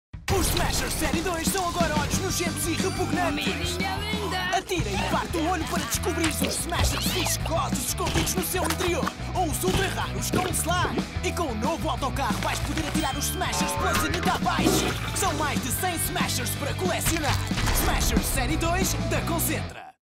SMASHERS SÉRIE 2 - OLHOS SURPRESA
Chegou a nova série dos Smashers! Agora as bolas smash são olhos nojentos que se atiram para revelar uma figura nojenta e pegajosa. São mais 100 novos personagens para colecionares, de 6 temas diferentes e edições raras e super raras para encontrares.